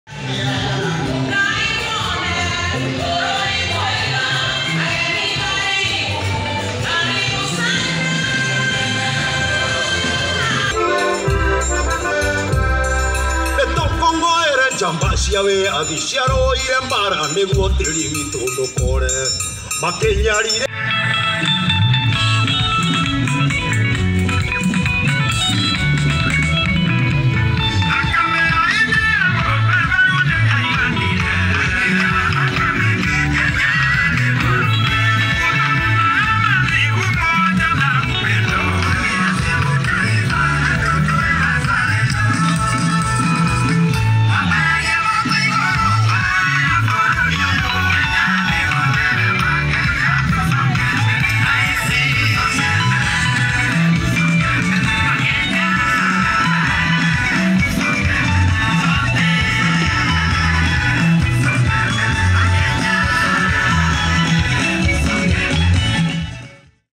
We are elated to enliven Mashujaa fete - artists
As the preparations for the Mashujaa Day fete hit a homestretch, various sets of groups have over the past two weeks pitched camp at the Wang’uru stadium in Kirinyaga County polishing their various items which they will be presenting before the President and other dignitaries.